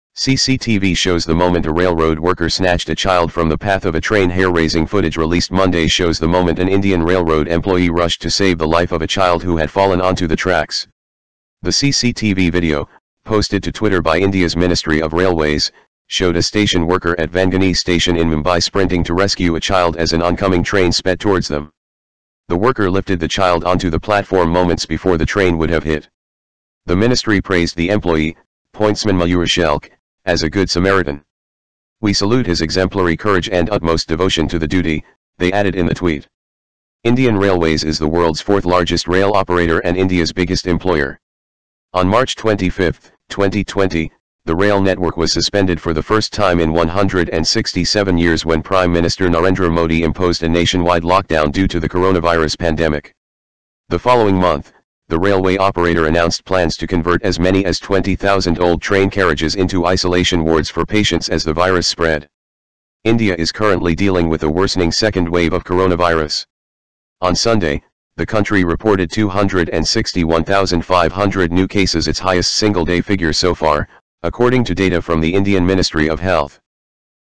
CCTV shows the moment a railroad worker snatched a child from the path of a train
(CNN)Hair-raising footage released Monday shows the moment an Indian railroad employee rushed to save the life of a child who had fallen onto the tracks.

The CCTV video, posted to Twitter by India's Ministry of Railways, showed a station worker at Vangani Station in Mumbai sprinting to rescue a child as an oncoming train sped towards them.

A Good Samaritan:

At Vangani station of Central Railway, Pointsman Mr. Mayur Shelkhe saved the life of a child just in the nick of the time. He risked his life to save the life of the child.

We salute his exemplary courage & utmost devotion to the duty.
 railroad worker snatched a child